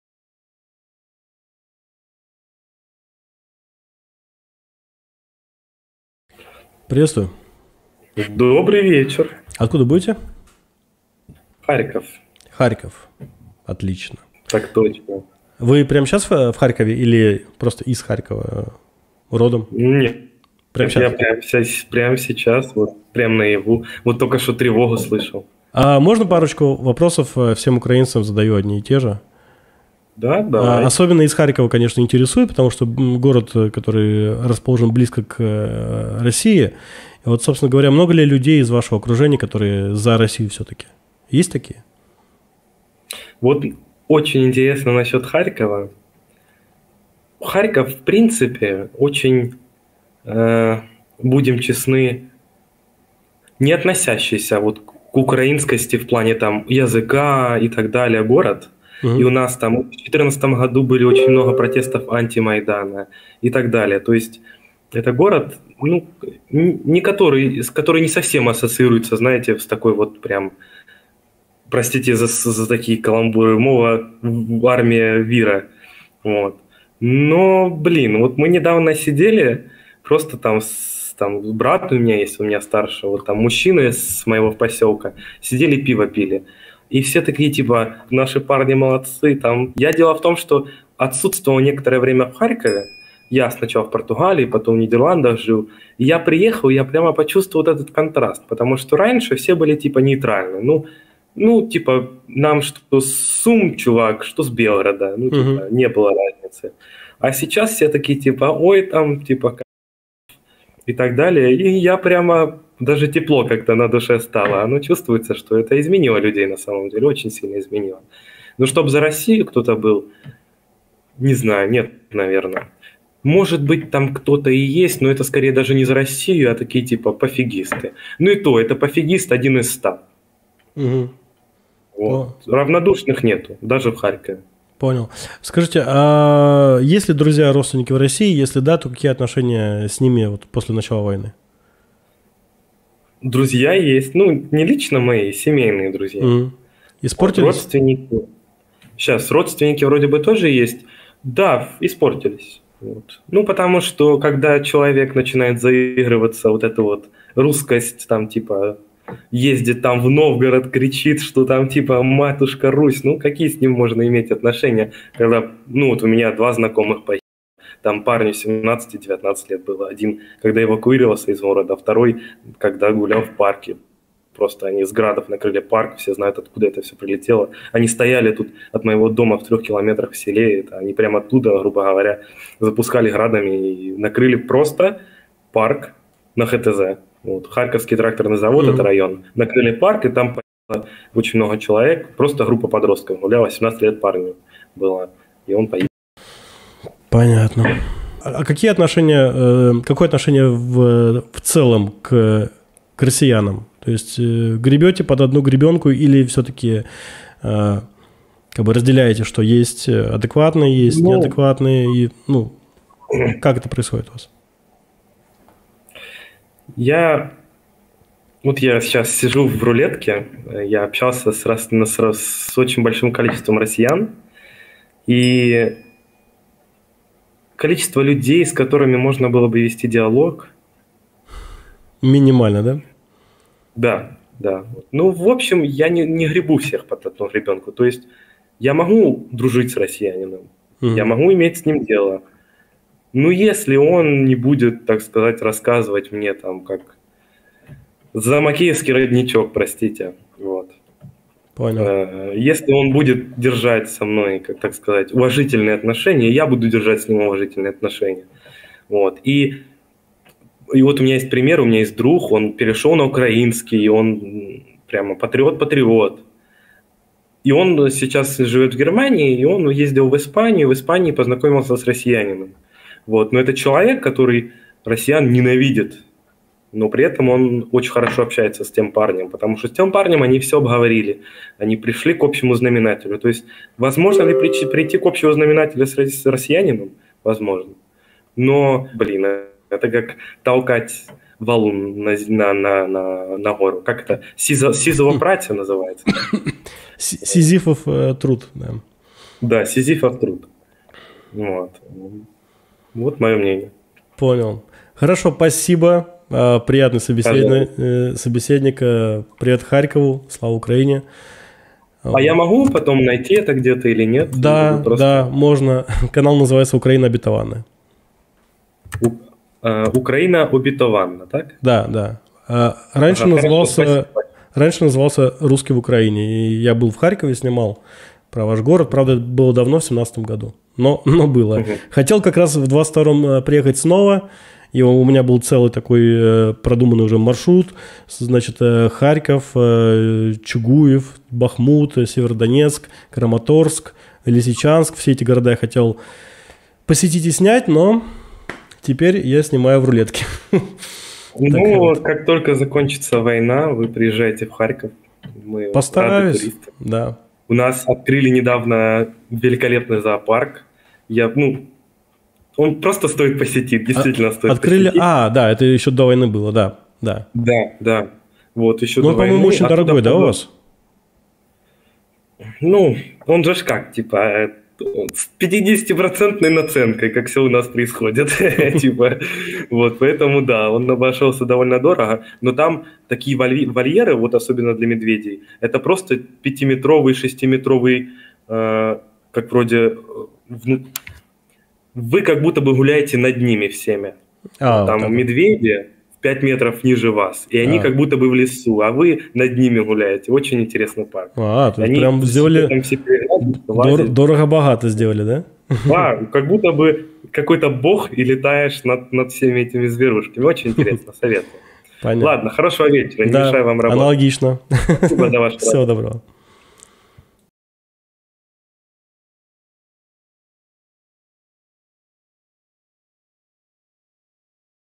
Харьковчанин о русских и отношении к ним. Украина Обетованная
💳 Сбербанк (по просьбе адекватных россиян, поддерживающих Украину): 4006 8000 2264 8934

📝КТО Я? Ответы на самые часто задаваемые вопросы